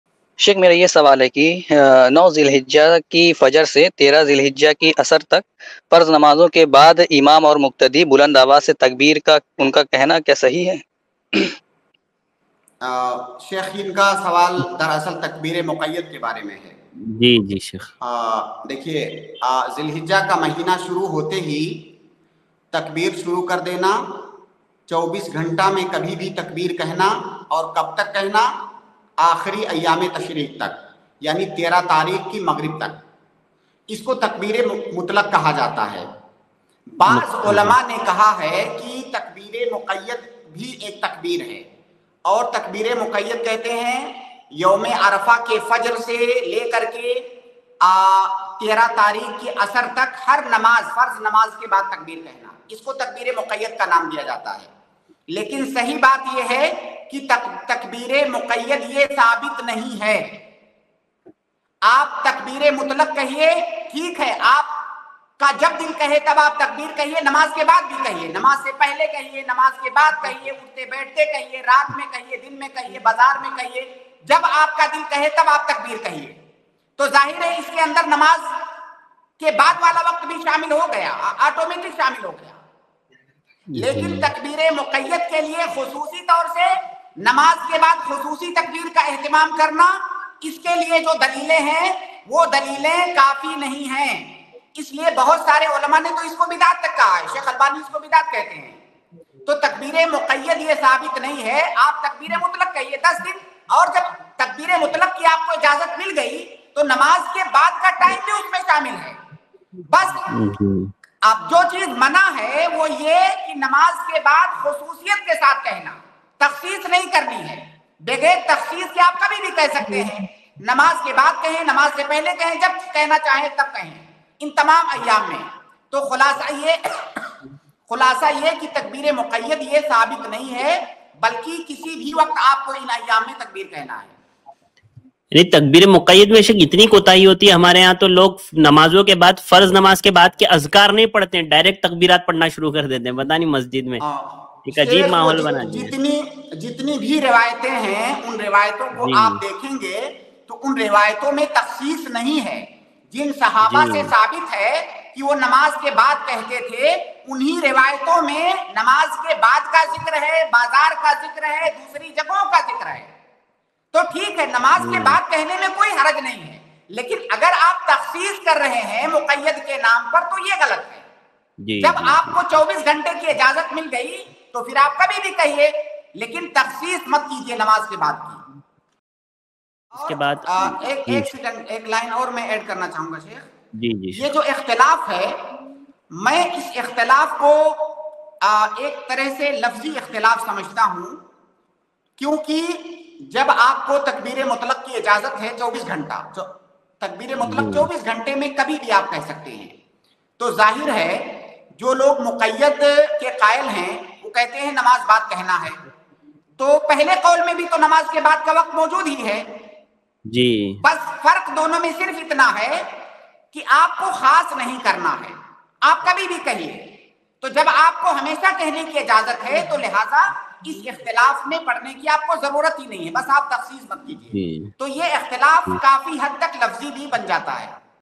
Zul Hijjah Mein 9 Se 13 Date Tak Namaz Ke Baad Buland Awaz Se Takbeer Kehna Kaisa Hai? By Dr. Naseem
Zul Hijjah Mein 9 Se 13 Date Tak Namaz Ke Baad Buland Awaz Se Takbeer Kehna Kaisa Hai? By Dr. Mohammad Naseem Madani iPlus TV Deeni Q&A

UPI : donateilm@icici